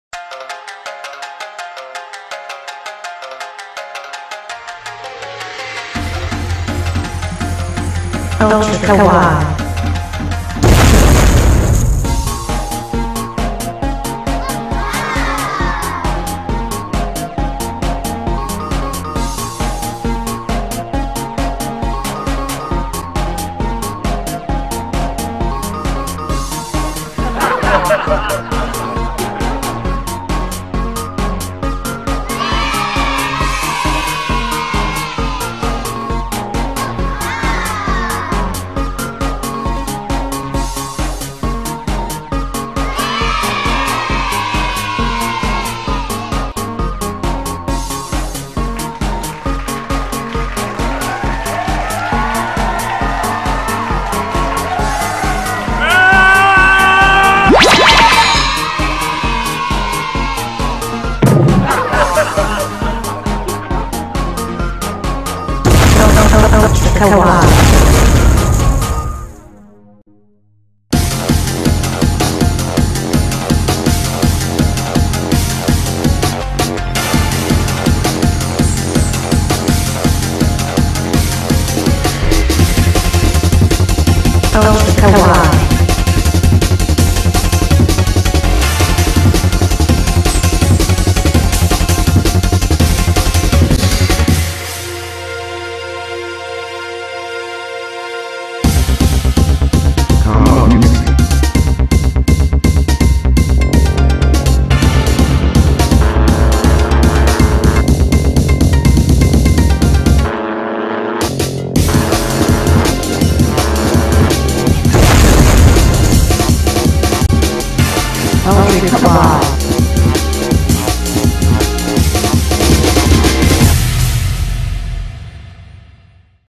Ultra Kawaii!!! Kittens, Corgis, Hedgehogs!
This is an early version we made a few months back of a new show that launches soon.  Hope you enjoy it -- let us know what you think. In this Ultra Kawaii: 6-Day Old Norwegian Kittens, Welsh Corgi Puppy, Hedgehogs, and KAMACUTESI